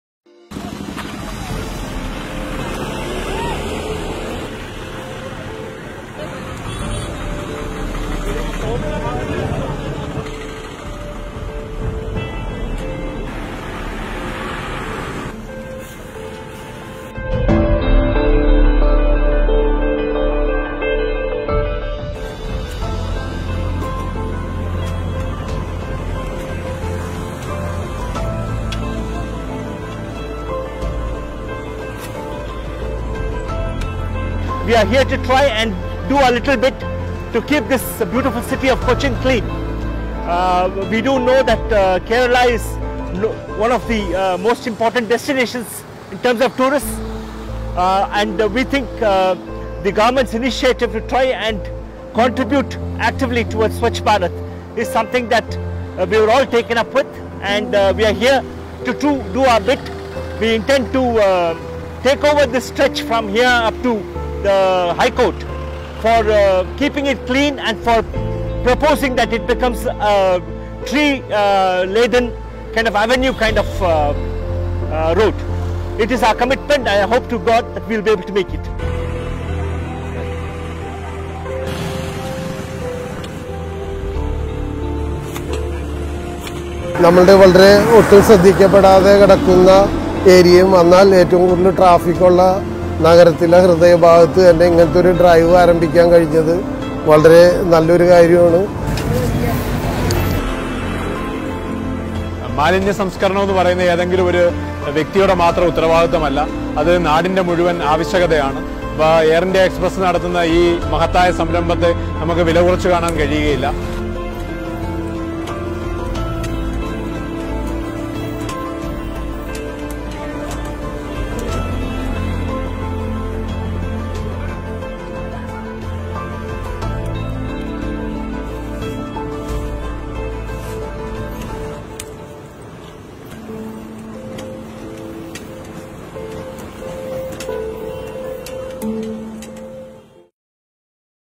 A SWACHH BHARAT initiative by Air India Express @ Kochi
On this Gandhi Jayanti, we were happy to share glimpses of a small initiaive of Air India Express.

Inspired by Mahatmas' vision, CEO Air India Express led the employees and National Service Scheme volunteers of Maharaja's college during the cleaning drive at the city of Kochi.

Thanks to Kochi corportation authorities for the support!!